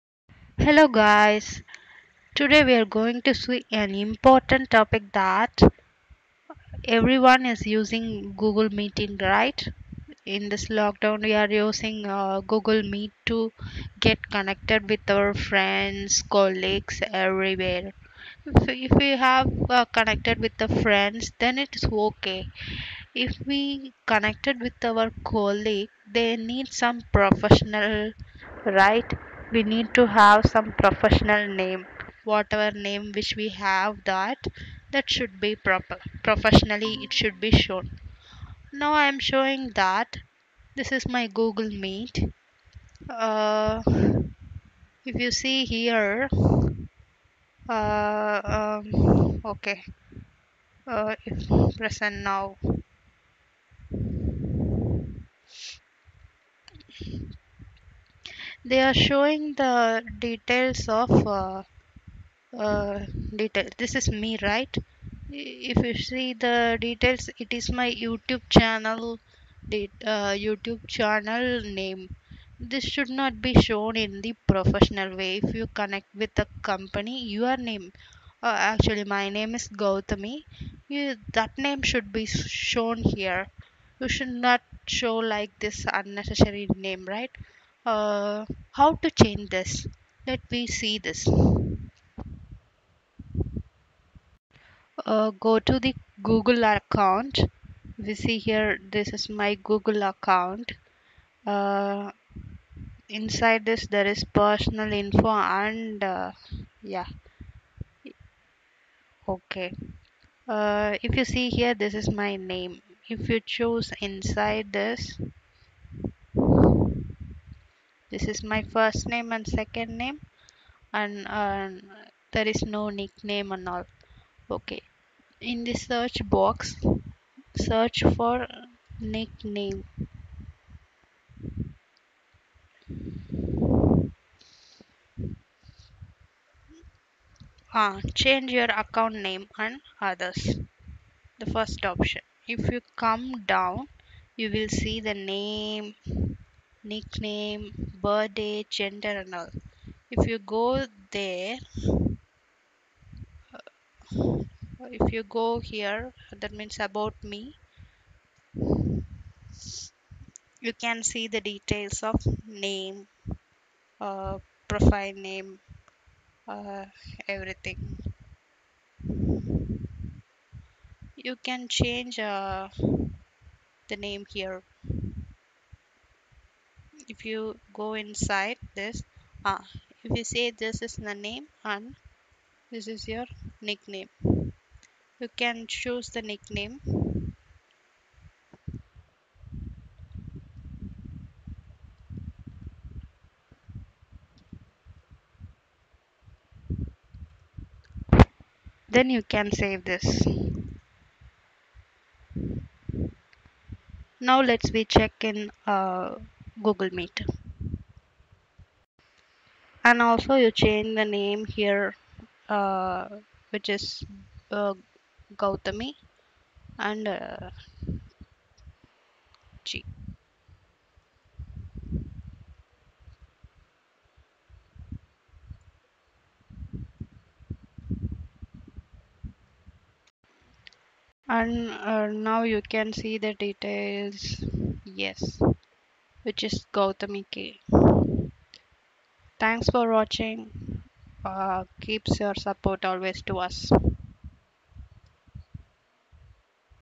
How to Change Name in Google Meet?
This video helps you to change the name in the Google meet

1)Sign in Google Account.

2)Go to the Google Account page.
3)Click Personal Info & Privacy into the Google Account page.
4)Scroll down the page and Click Name.
5)Click Pencil Icon Next to your Name.
6)Change the Name and Click Done.